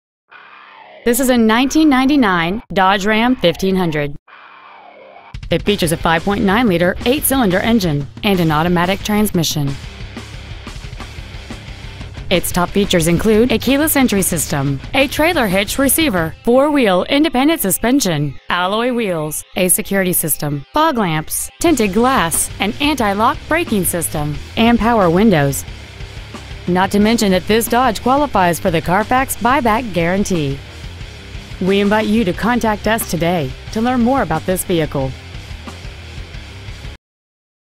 1999 Dodge Ram 1500 Stone Mountain GA 30087
Year : 1999
 Make : Dodge
 Model : Ram 1500
 Engine : 5.9L V-8 cyl
 Trans . : Automatic
 Exterior : Green
 Miles : 124,695

 5065 U.S. Highway 78
 Stone Mountain , GA 30087
 Used 1999 Dodge Ram 1500 Stone Mountain GA